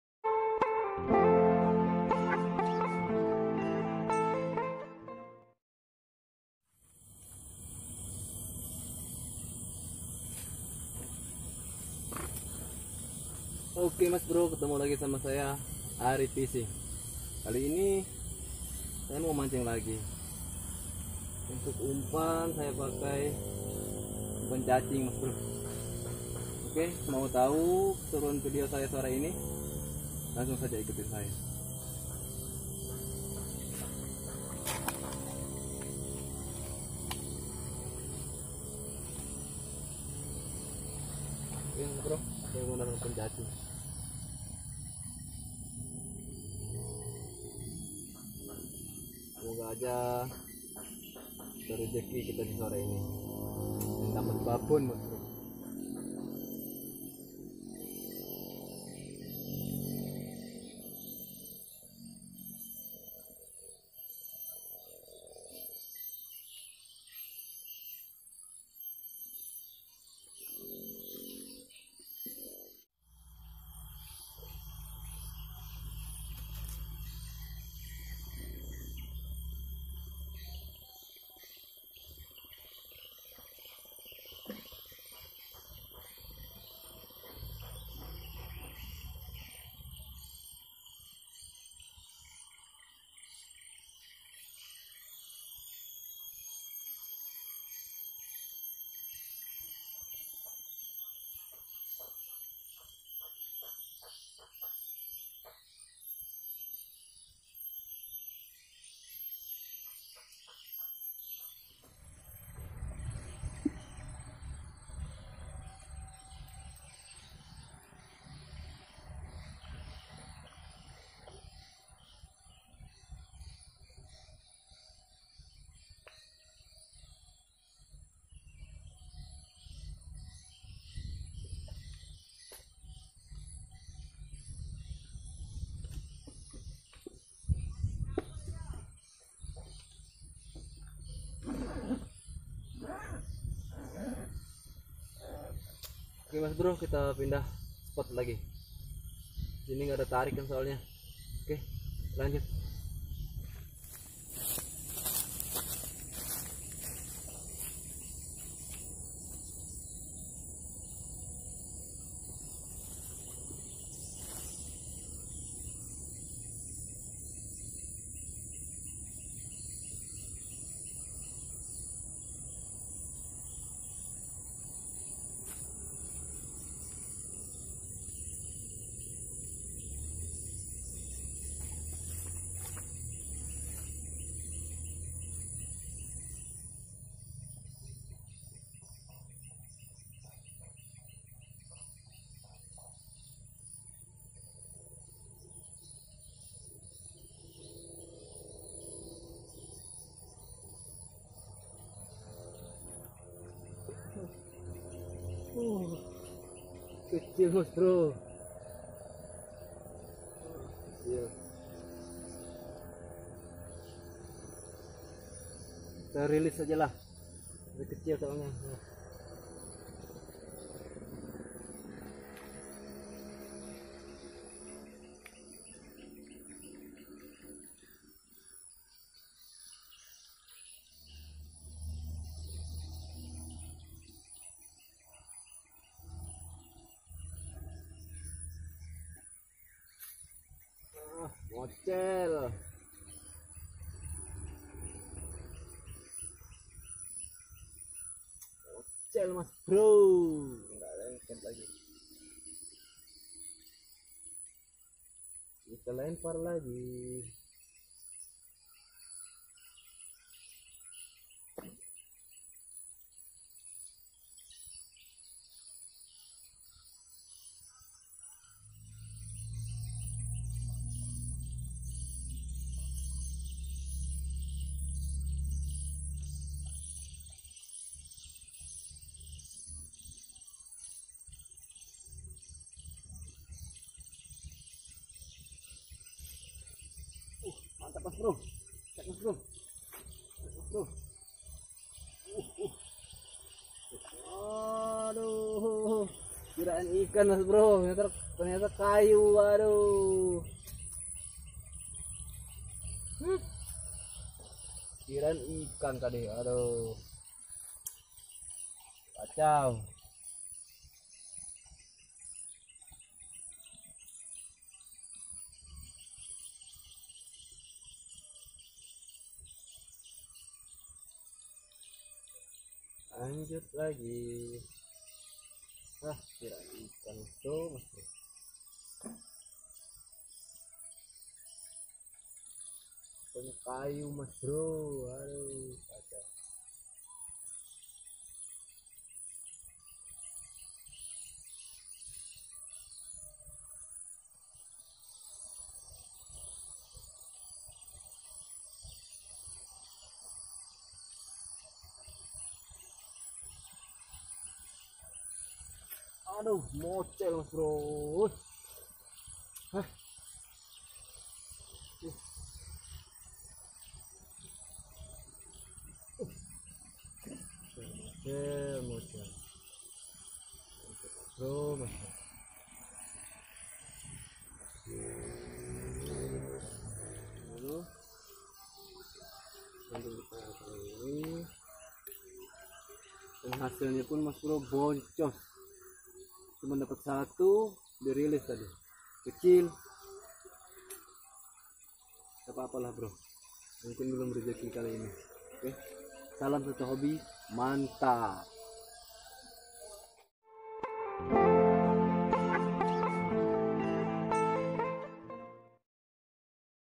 mancing ikan di sungai_di kira dapat babon ternyata malah dapat
Assalamualaikum sahabat mancing di Mana pun berada berjumpa lagi sama saya Arie fishing. seperti biasa saya mau mancing lagi untuk pancing yang saya guna kan pancing dasar an umpan nya saya guna kan cacing gimana keseruan nya lansung aja ikut in saya